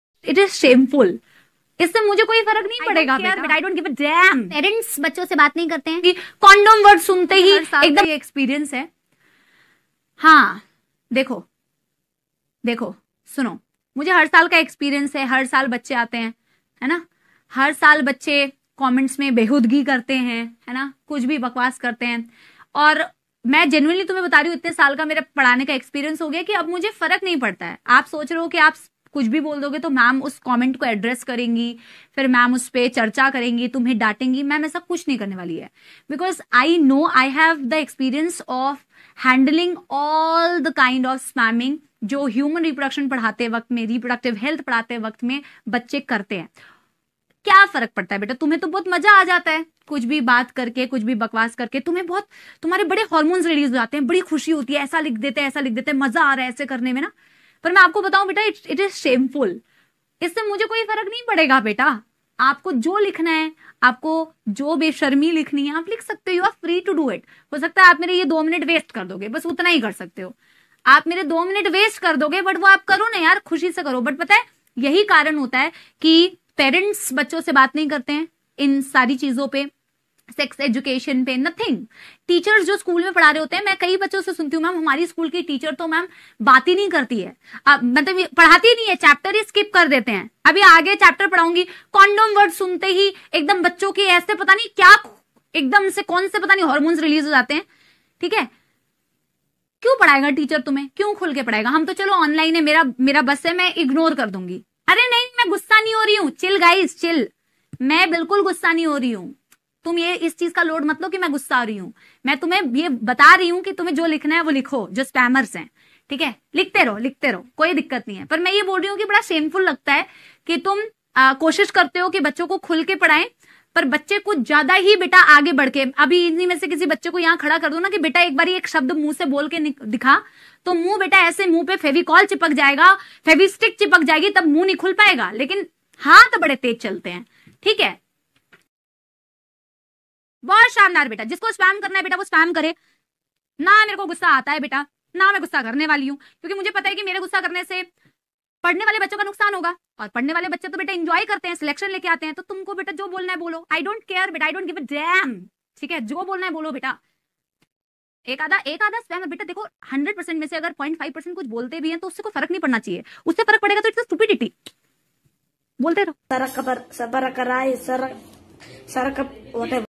Samapti Mam angry on students 🤬!! Reproduction practical 😤Lakshya NEET 2025 live class !!
Lakshya NEET 2025 live batch Samapti maam reaction on comments of some careless students saying about reproduction practical🤬 !!! ANGRY😤😤 MOMENT

🛑NOTE:- This is not an official channel of physics wallah alakh pandey sir

• Watch